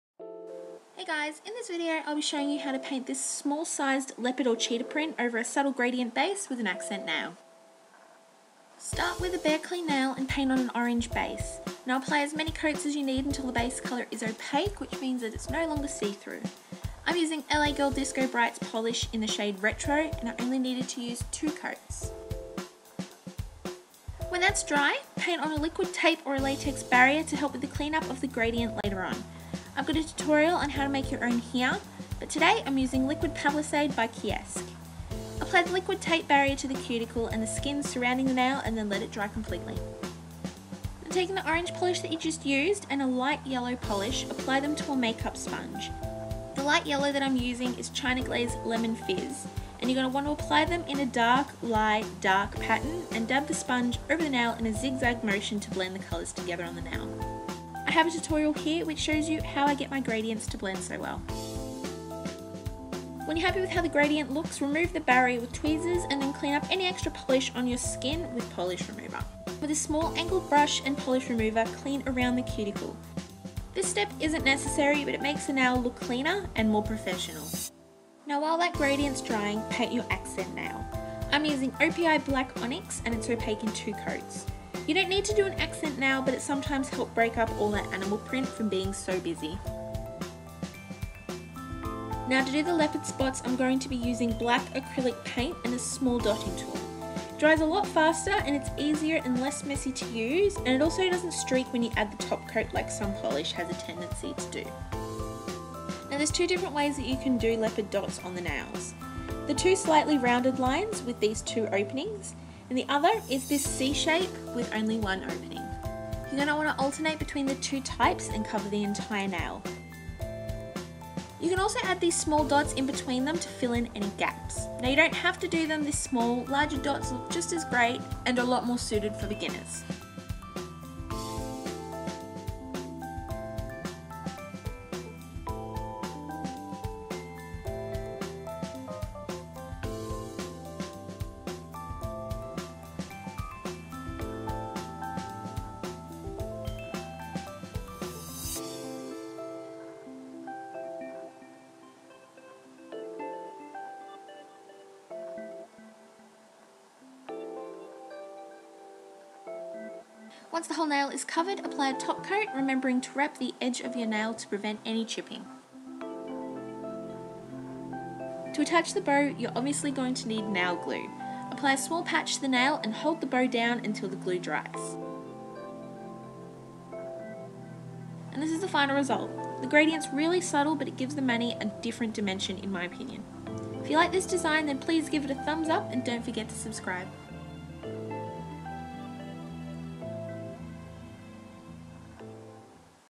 Leopard Print Nails (With A Gradient!)
I'm not the biggest fan of leopard/cheetah print, unless it's on my nails and then I LOVE it!!

In this video I'll show you how to recreate this simple leopard print over a subtle gradient design. The gradient helps turn this from a normal animal print mani to something next level.

I also added a 'no feature' nail since it's technically not a feature nail being so plain next to the leopard print, is it?

All you need it a makeup sponge, a dotting tool or bobby pin/dressmaker pin and the bow, if you choose to add one, to complete this look!

A full list of the products used below:

Makeup sponge from Daiso
Pink bow from eBay
Dotting tool from eBay
Black acrylic paint from my local newsagent/craft store